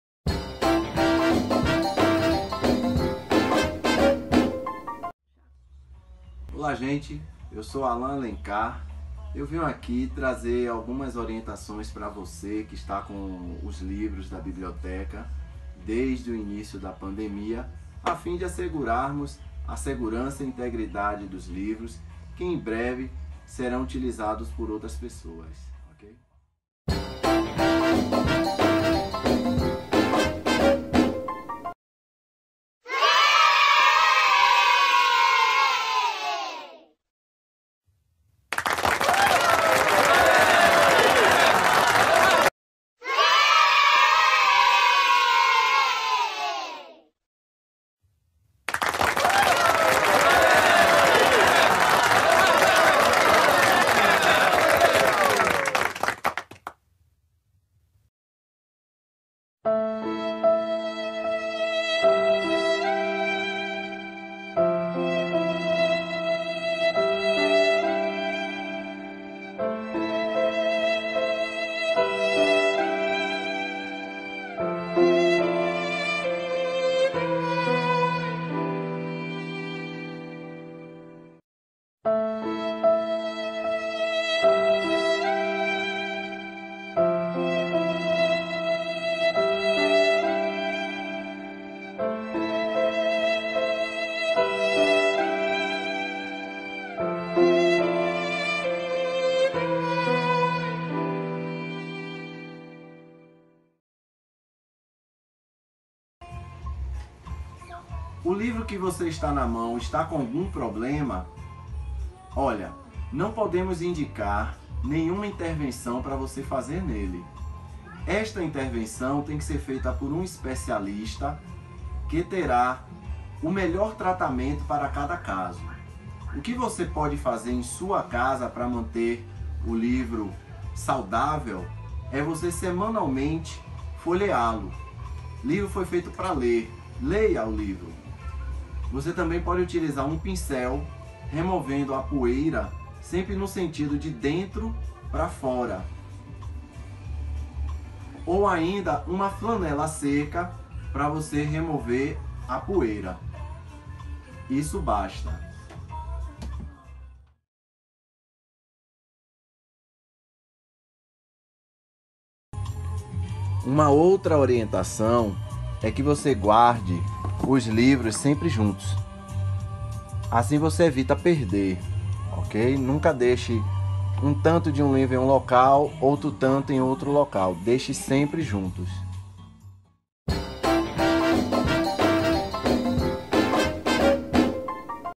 Cuidando dos livros na pandemia (e após).
Cuidados que os leitores devem ter com os livros da biblioteca no período da pandemia. Sobretudo nas situações em que o leitor está a vários meses com os livros em casa.
Como manter a conservação dos livros.
Como guardar os livros adequadamente.